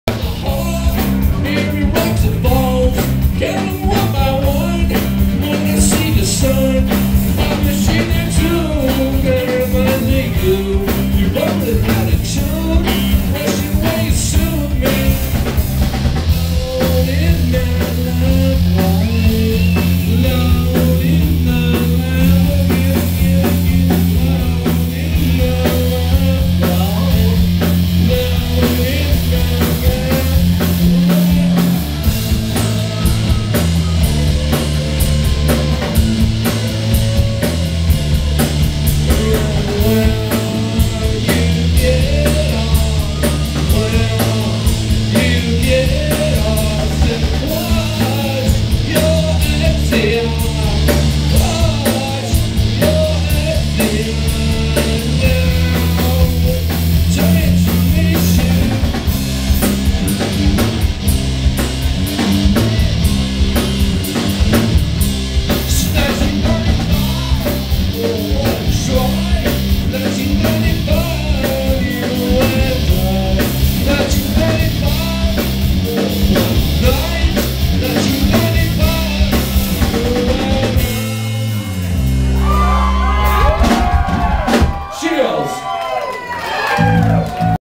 Starsky @ Launchpad 10/15/10 Gary Wayne Nelson Benefit
Oops--missed the first few seconds of the song. Camera pans to see a 140.5 decibel reading, then wanders slightly off-target while I had one of those "OMG, I haven't seen you in 20 years!" moments...